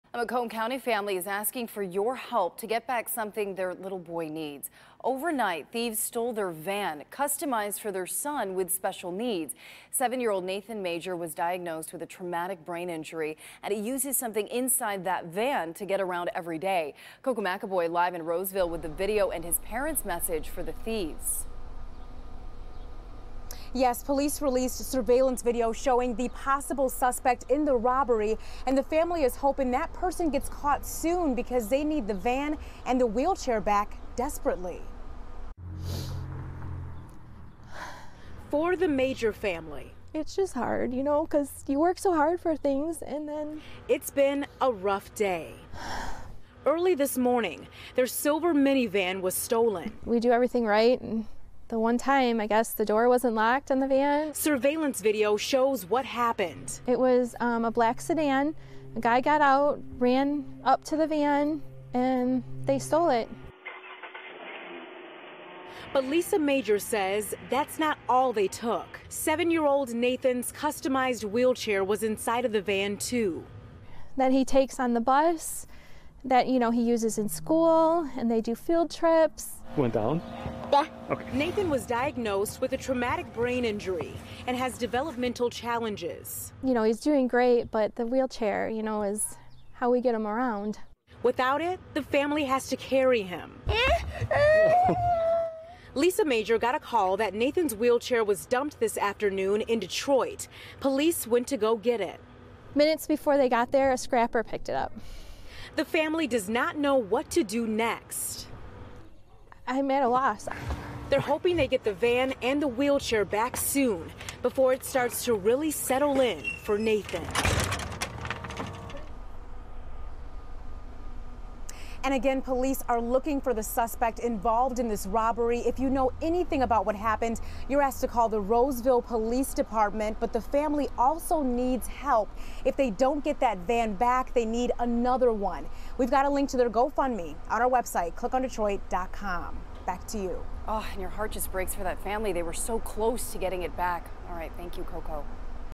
Van, wheelchair stolen from boy
A Macomb County family is asking for help in order to get back a stolen van that is used for their son with special needs.